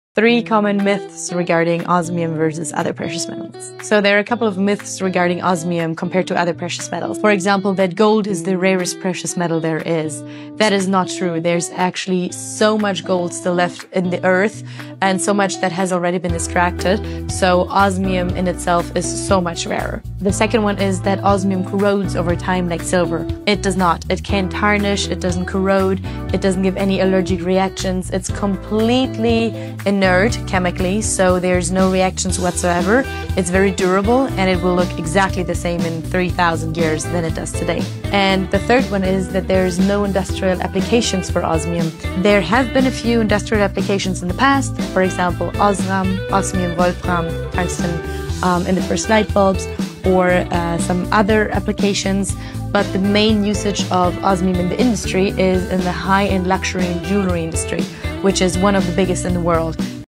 3 common myths regarding Osmium - The most precious, densest and rarest metal - The SUNSHINE ELEMENT
3 common myths regarding Osmium